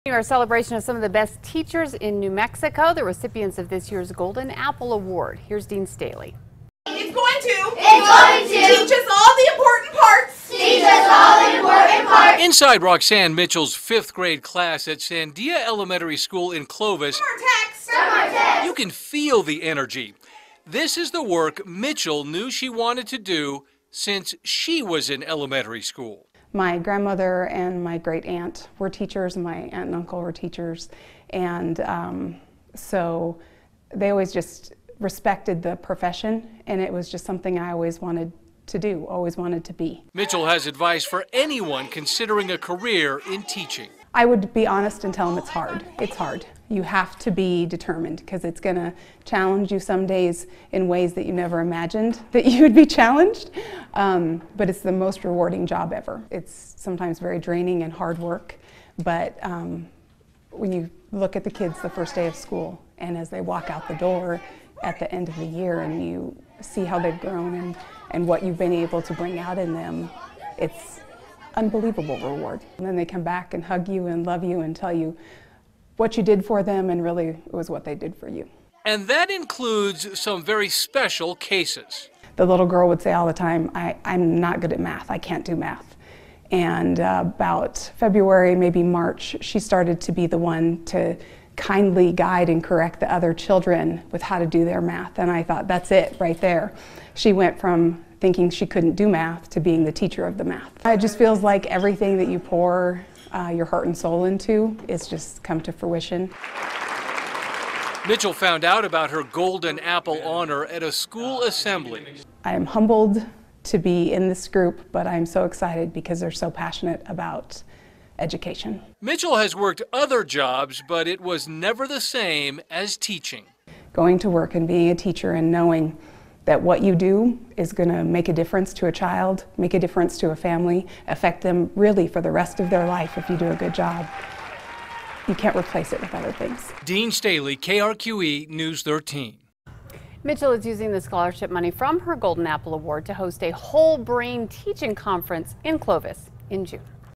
Golden Apple: Roxanne Mitchell teaching to make a difference in children's lives
Golden Apple: Roxanne Mitchell teaching to make a difference in children's lives - Source